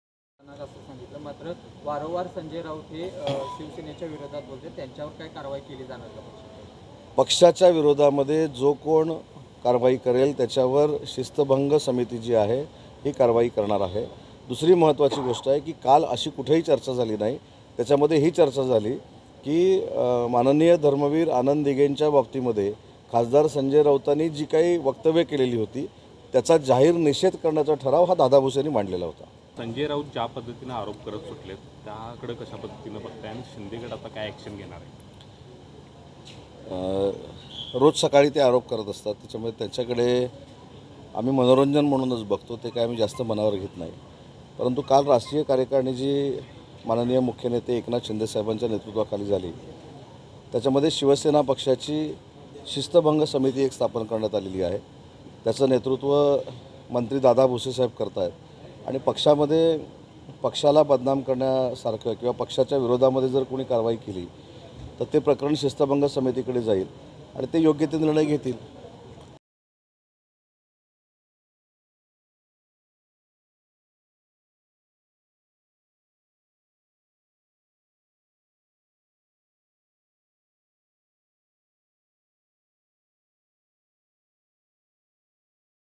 Uday Samant On Sanjay Raut : सामंतांचा संजय राऊतांना टोला | LetsUpp Marathi
संजय राऊत यांच्याकडे आम्ही मनोरंजन म्हणून पाहतो. असा टोला मंत्री उदय सामंत यांनी संजय राऊतांना लगावला आहे.